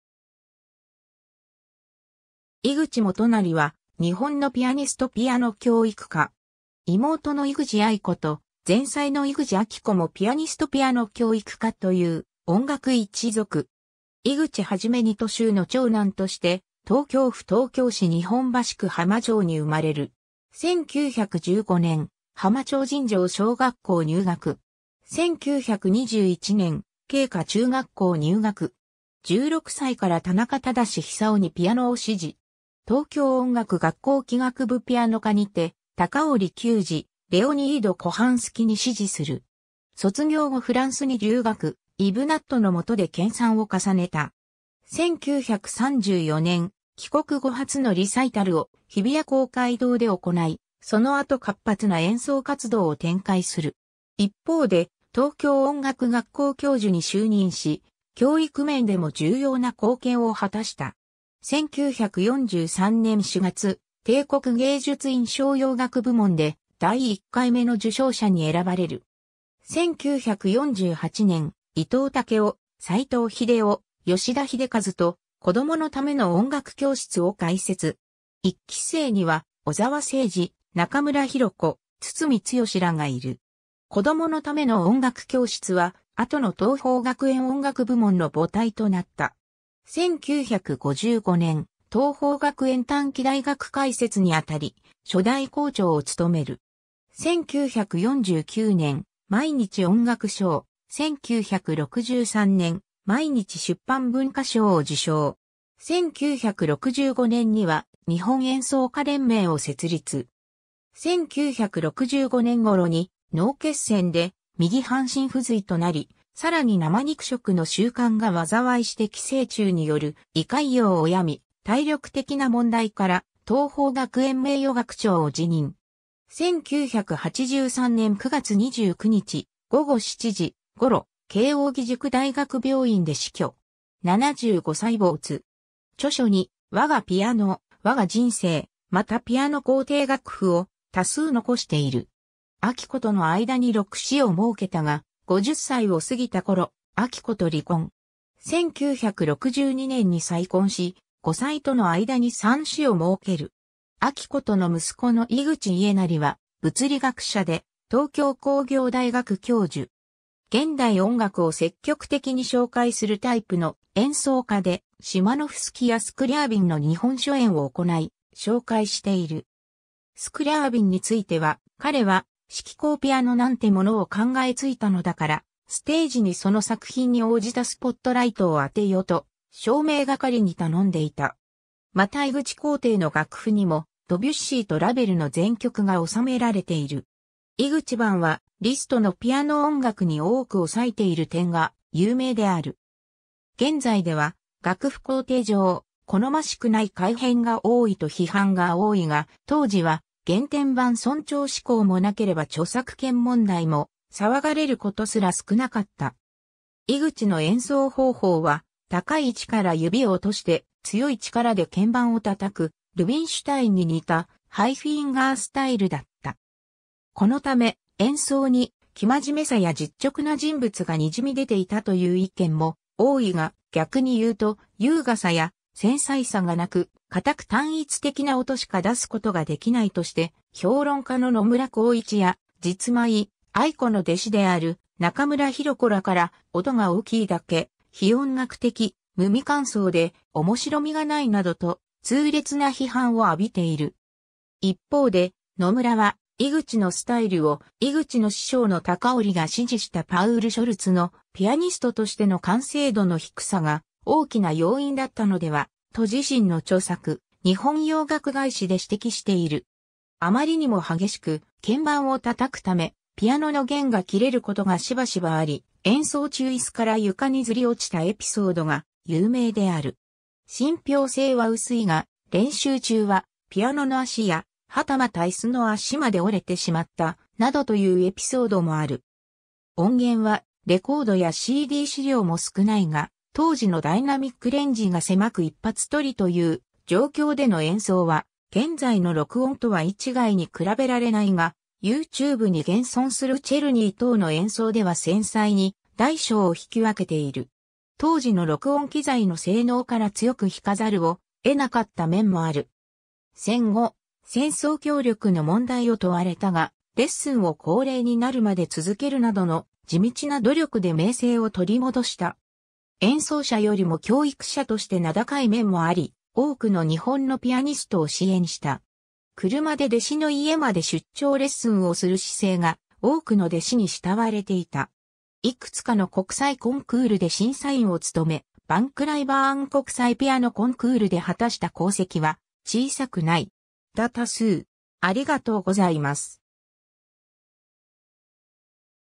井口基成
井口 基成（いぐち もとなり、1908年5月17日 - 1983年9月29日）は日本のピアニスト・ピアノ教育家。
妹の井口愛子（後に佐藤愛子）と、前妻の井口秋子もピアニスト・ピアノ教育家という音楽一族。
 井口基二と秀の長男として東京府東京市日本橋区浜町（現東京都中央区日本橋浜町）に生まれる。
 1915年、浜町尋常小学校入学。
1921年、京華中学校入学。
16歳から田中規久士にピアノを師事。
東京音楽学校器楽部ピアノ科にて高折宮次、レオニード・コハンスキに師事する。
卒業後フランスに留学、イヴ・ナットのもとで研鑽を重ねた。
1934年、帰国後初のリサイタルを日比谷公会堂で行い、その後活発な演奏活動を展開する。
一方で東京音楽学校教授に就任し、教育面でも重要な貢献を果たした。
 1943年4月、帝国芸術院賞洋楽部門で、第1回目の受賞者に選ばれる。
1948年、伊藤武雄、齋藤秀雄、吉田秀和と「子供のための音楽教室」を開設。
一期生には小澤征爾（指揮者）、中村紘子（ピアニスト）、堤剛（チェリスト）らがいる。
「子供のための音楽教室」は、後の桐朋学園音楽部門の母体となった。
1955年、桐朋学園短期大学開設にあたり、初代校長をつとめる。
 1949年、毎日音楽賞、1963年、毎日出版文化賞を受賞。
1965年には日本演奏家連盟を設立。
 1965年頃に脳血栓で右半身不随となり、さらに生肉食の習慣が災いして寄生虫による胃潰瘍を病み、体力的な問題から桐朋学園名誉学長を辞任。
 1983年9月29日、午後7時頃、慶應義塾大学病院で死去。
75歳没。
 著書に「わがピアノ、わが人生」、またピアノ校訂楽譜を多数残している。
 秋子との間に6子を儲けたが、50歳を過ぎた頃、秋子と離婚。
1962年に再婚し、後妻との間に3子を儲ける。
秋子との息子の井口家成は物理学者で東京工業大学教授。
 現代音楽を積極的に紹介するタイプの演奏家で、シマノフスキやスクリャービンの日本初演を行い、紹介している。
スクリャービンについては「彼は色光ピアノなんてものを考え付いたのだから、ステージにその作品に応じたスポットライトを当てよ」と照明係に頼んでいた。
また井口校訂の楽譜（井口版）にもドビュッシーとラヴェルの全曲が収められている。
井口版はリストのピアノ音楽に多くを割いている点が有名である。
現在では楽譜校訂上好ましくない改変が多いと批判が多いが、当時は原典版尊重志向もなければ著作権問題も騒がれることすら少なかった。
 井口の演奏方法は、高い位置から指を落として強い力で鍵盤を叩く、ルビンシュタインに似た「ハイフィンガー・スタイル」だった。
このため、演奏に生真面目さや実直な人物がにじみ出ていたという意見も多いが、逆に言うと優雅さや繊細さがなく、固く単一的な音しか出すことができないとして、評論家の野村光一や、実妹・愛子の弟子である中村紘子らから「音が大きいだけ」「非音楽的」「無味乾燥で面白みがない」などと痛烈な批判を浴びている（中村本人の演奏に対しても全く同様の批評が与えられる傾向がある）。
一方で野村は、井口のスタイルを、井口の師匠の高折が師事したパウル・ショルツのピアニストとしての完成度の低さ（高折が師事した当時、ショルツは音楽院をでたばかりの24歳であった）が大きな要因だったのでは、と自身の著作『日本洋楽外史』で指摘している。
 あまりにも激しく鍵盤を叩くため、ピアノの弦が切れることがしばしばあり、演奏中椅子から床にずりおちたエピソードが有名である。
信憑性は薄いが、練習中はピアノの脚や、はたまた椅子の脚まで折れてしまった、などというエピソードもある。
 音源はレコードやCD資料も少ないが当時のダイナミックレンジが狭く一発録りという状況での演奏は現在の録音とは一概に比べられないが、youtubeに現存するチェルニー等の演奏では繊細に大小を弾き分けている。
当時の録音機材の性能から強く弾かざるを得なかった面もある。
 戦後、戦争協力の問題を問われたが、レッスンを高齢になるまで続けるなどの、地道な努力で名声を取り戻した。
演奏者よりも教育者として名高い面もあり、多くの日本のピアニストを支援した。
車で弟子の家まで出張レッスンをする姿勢が、多くの弟子に慕われていた。
いくつかの国際コンクールで審査員を務め、ヴァ...